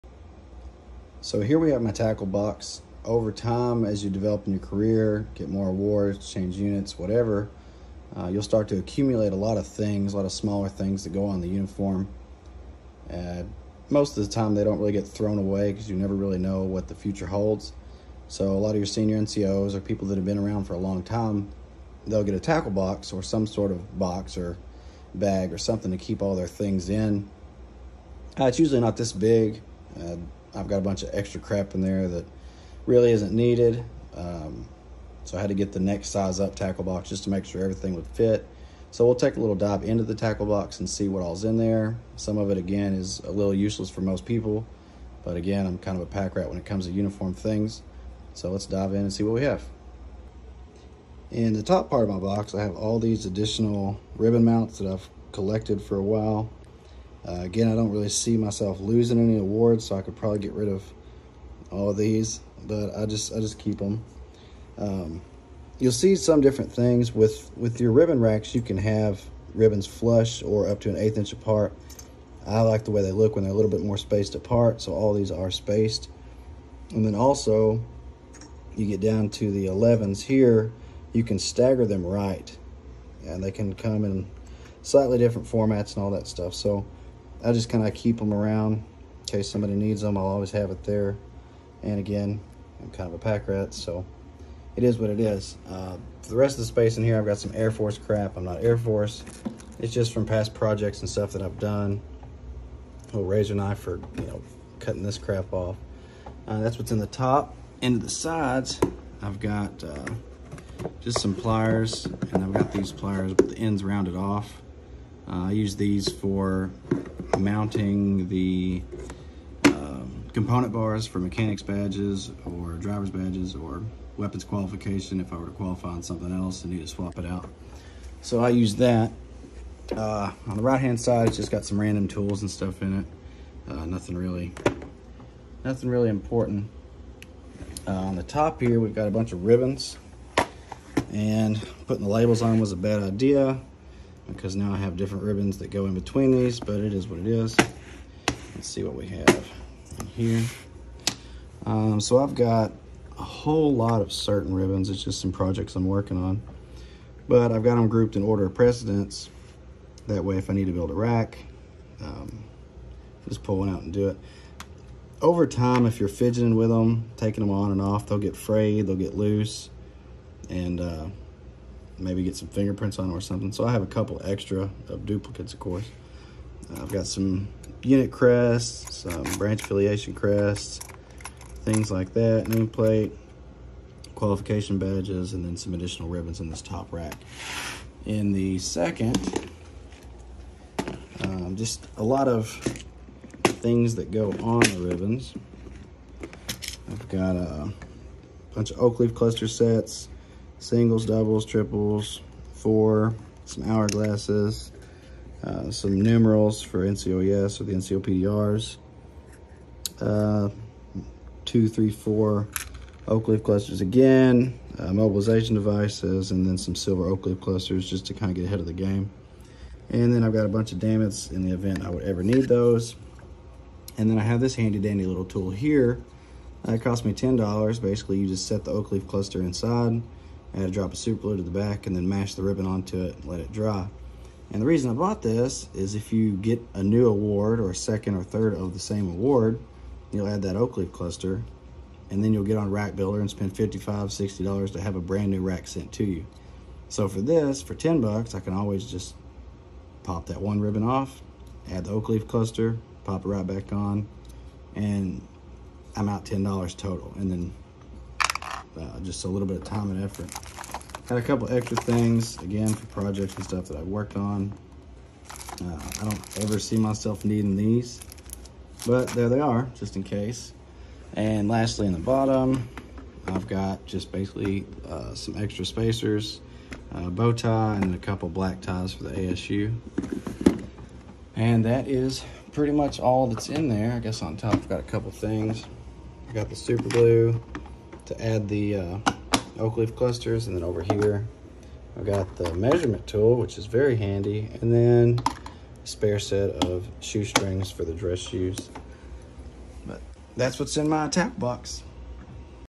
What’s in my tackle box?
Having a look at all the useless crap in my tackle box.